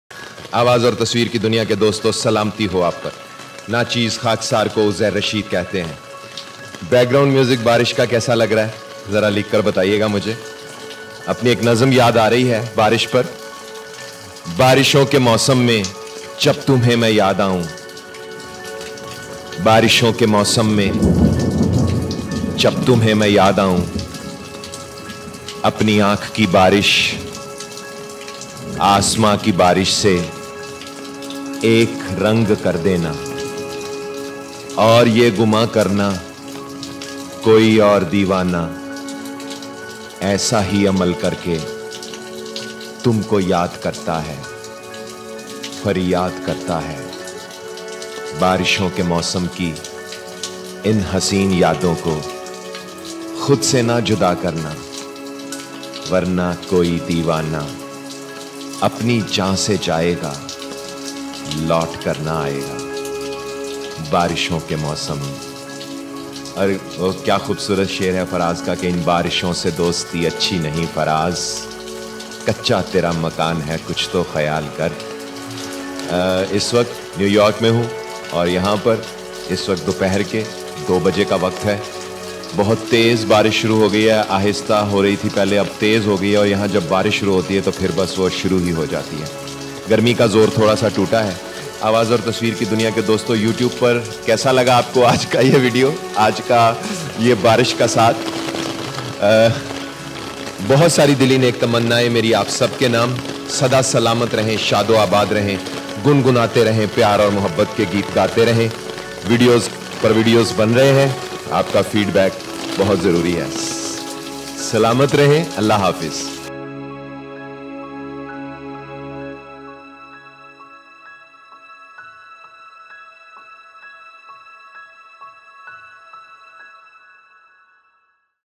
Barish : Poem By @UzairRashid │ Urdu Poetry │ Hindi Poetry
Heart Touching Poetry By @UzairRashid
عُزیررشید کی خوبصورت آواز سے  دلوں پر دستک

Content Written & Narrated by: Uzair Rashid